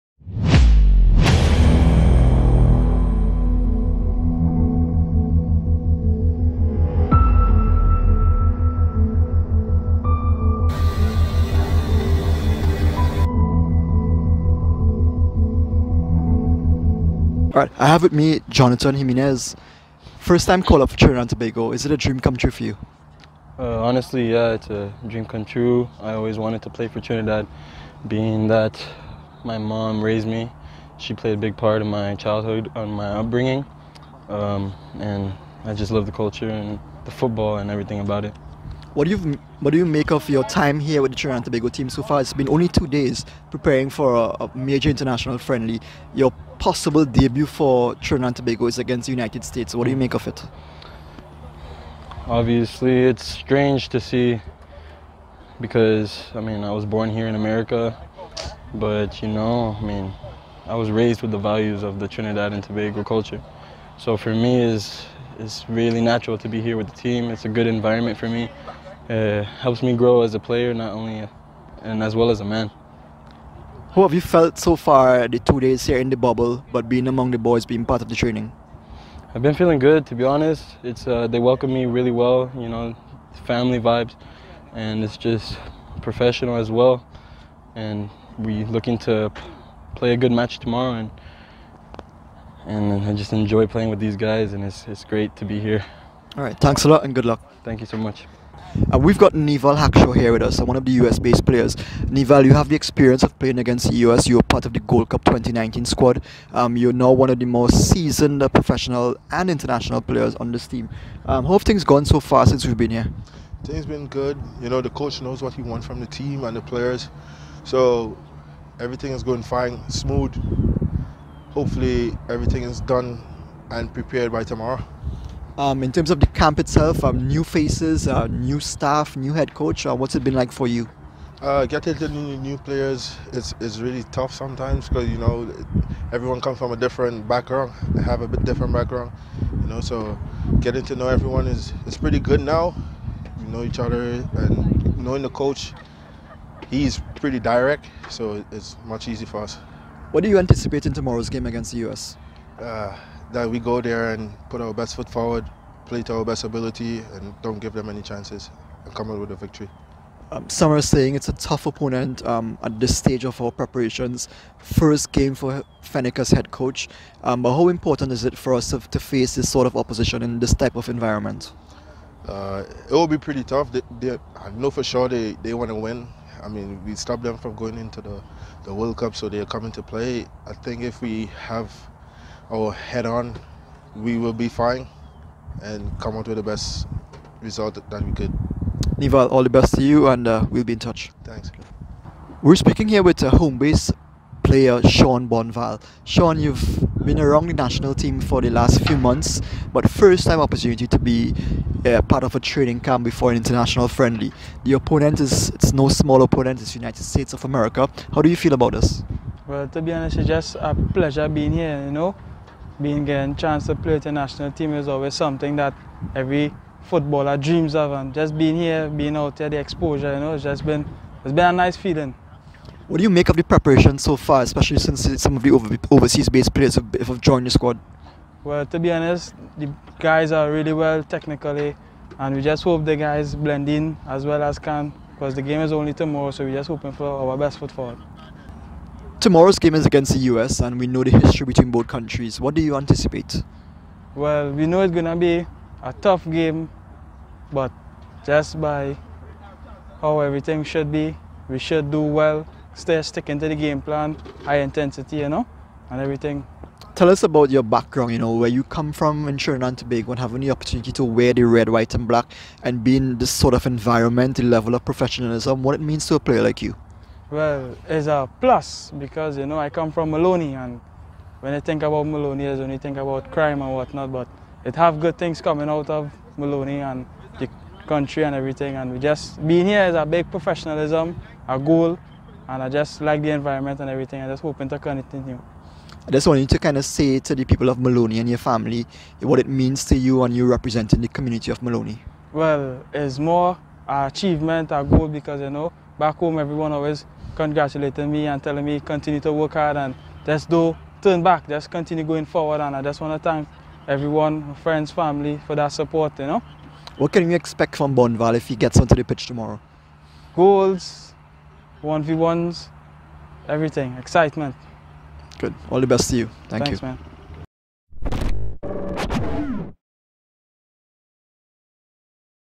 T&T vs USA Player Pre-Match Comments with Hackshaw, Jiminez and Bonval
T&T trio Neveal Hackshaw, Sean Bonval and US-based Jonathan Jiminez look ahead to USA Friendly - TTFA Media TV